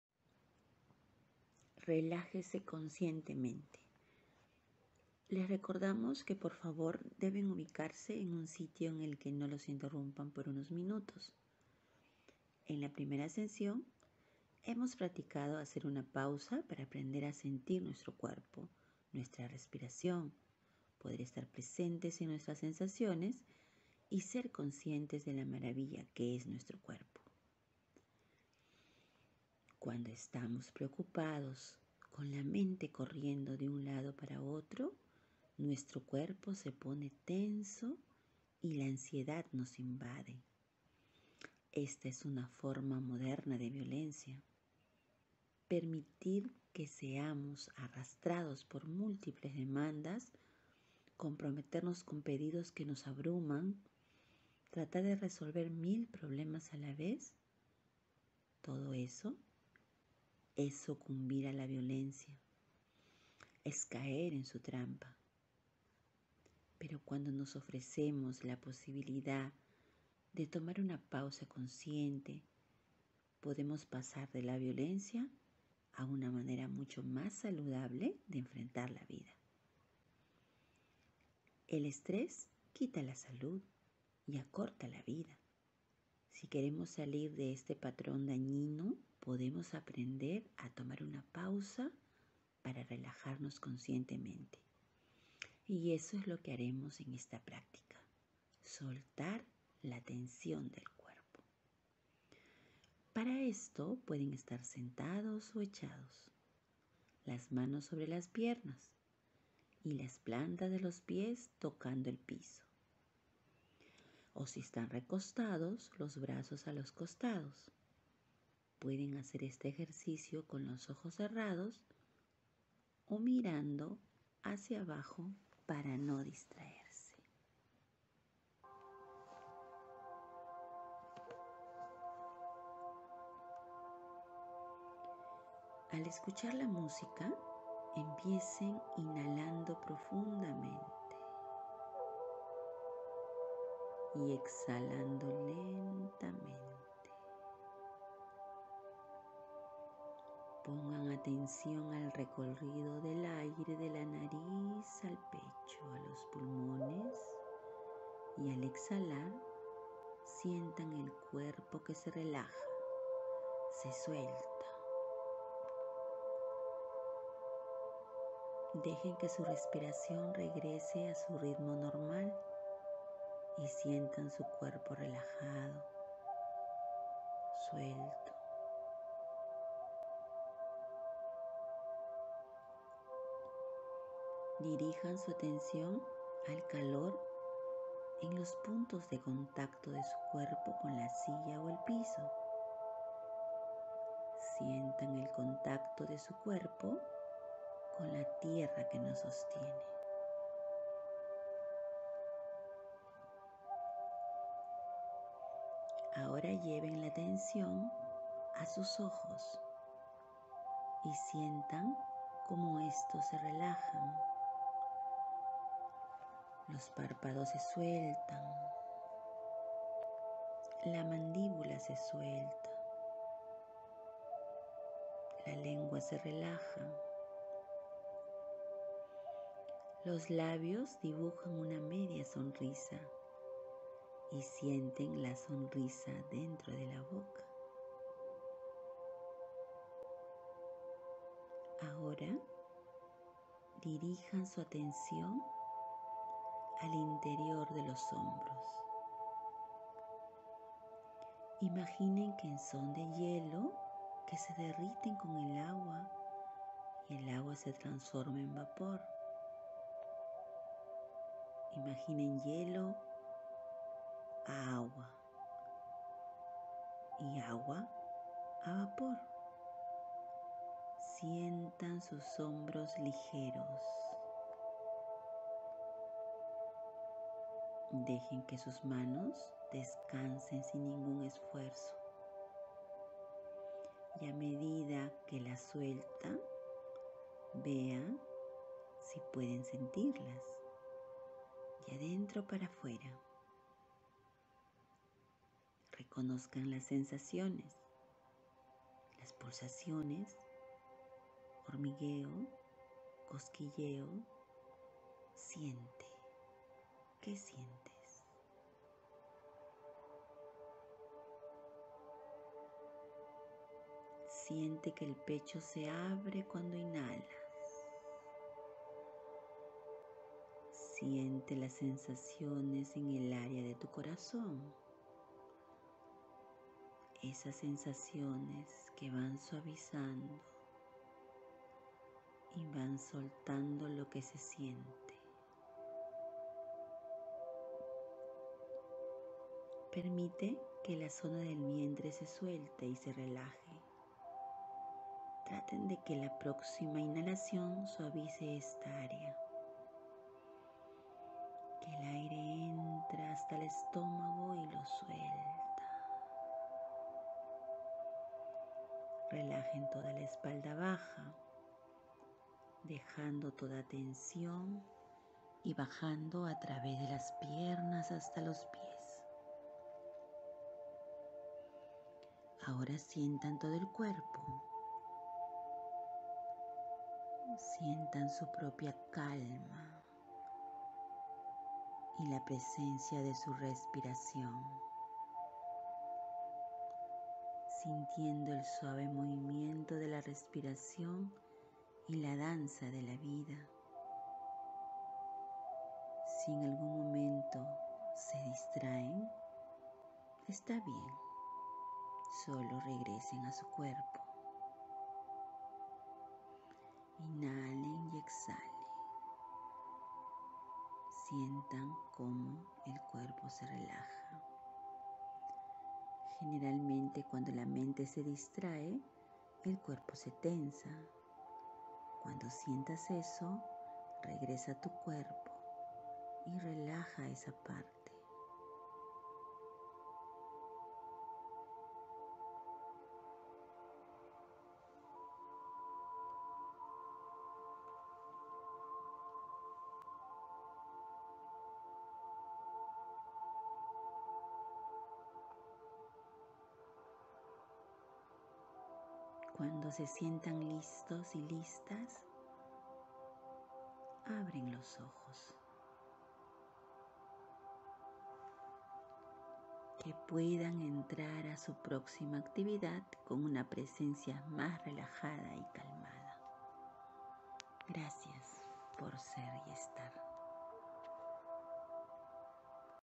6.- Descubro mi respiración
Programa La Magia del círculo: Prácticas de respiración consciente para que disminuyas el estrés, encuentres la calma y descubras las cualidades de tu corazón como la gratitud, el autocuidado, el respeto, la tolerancia, el perdón y la confianza para tu bienestar físico y emocional.